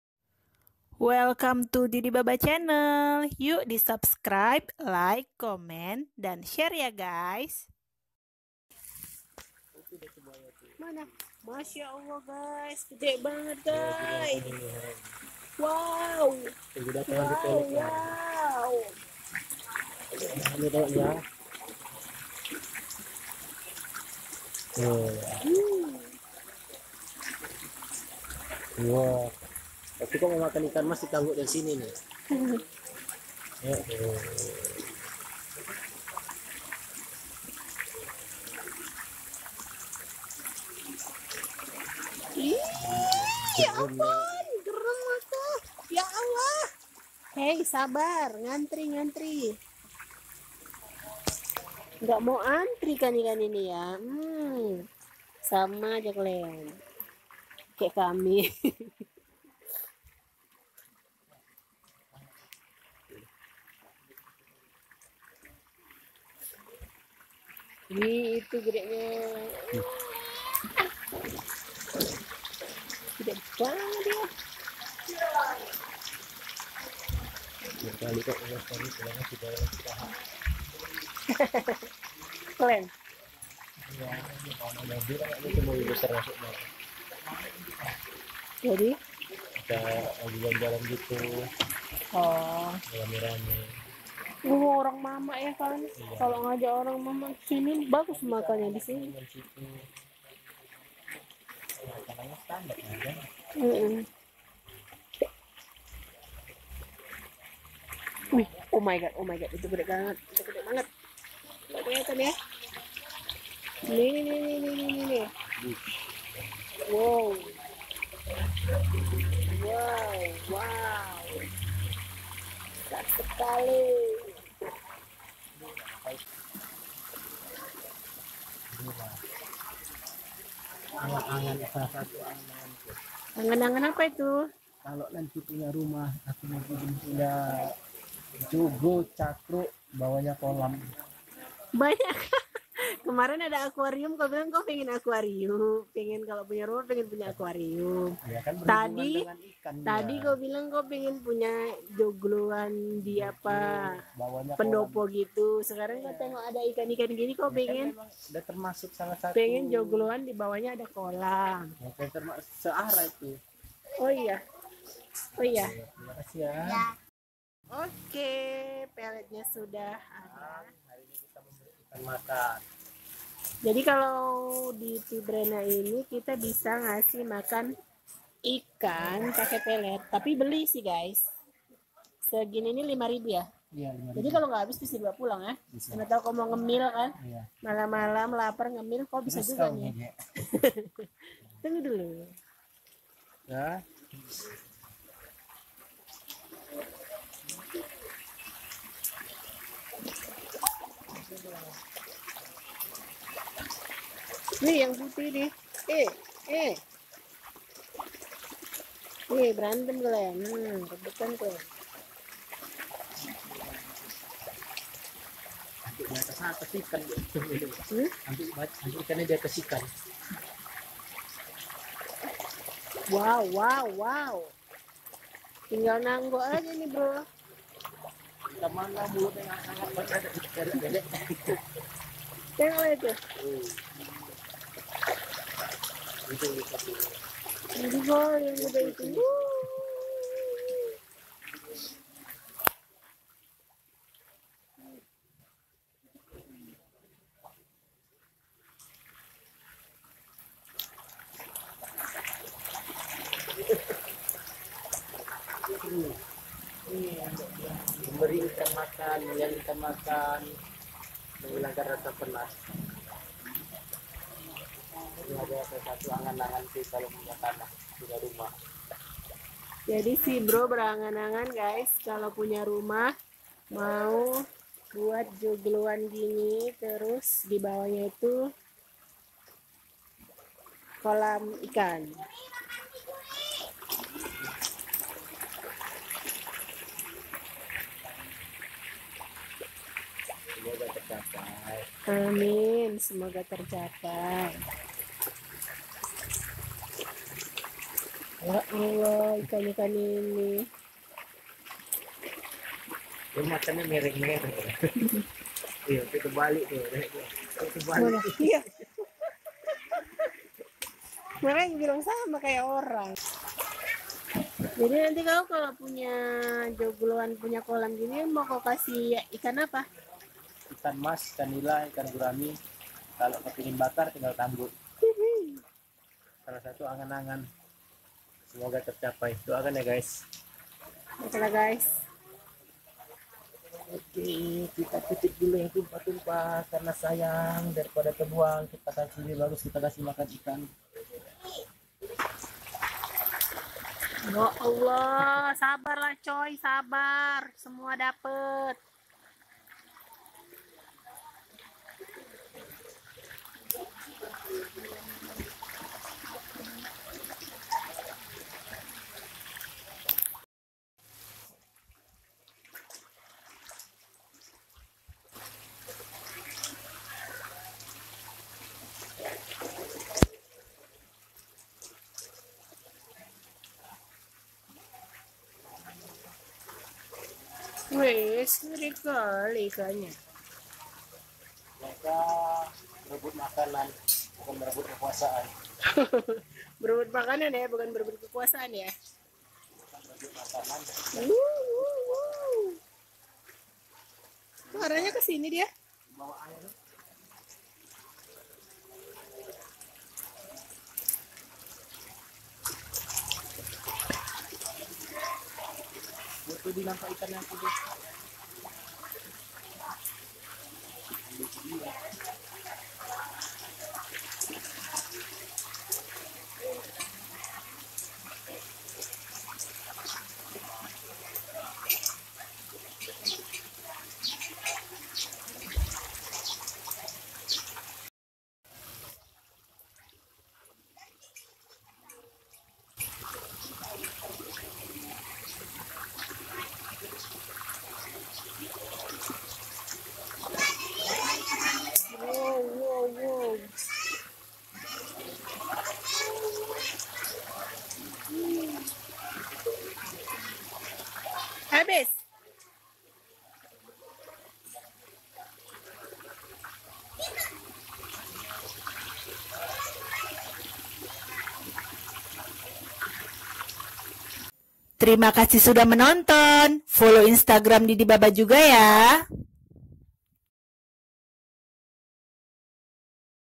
Serunya Si Bro Ngasih Makan Ikan Mas Sambil Berangan-angan
Memberi makan ikan ternyata memiliki keasyikan tersendiri. Apalagi jika ikannya banyak, bisa dipastikan mereka bakal berebut dan suana jadi seru karenanya.

Si Bro kemarin waktu ke Lesehan Terapung Tibrena Sibolangit langsung kepengen ngasih makan ikan mas begitu nyampe lokasi. Sambil ngasih makan ikan-ikan mas itu, dia berandai-andai berbagai hal :D

Support juga sosmed kami ya!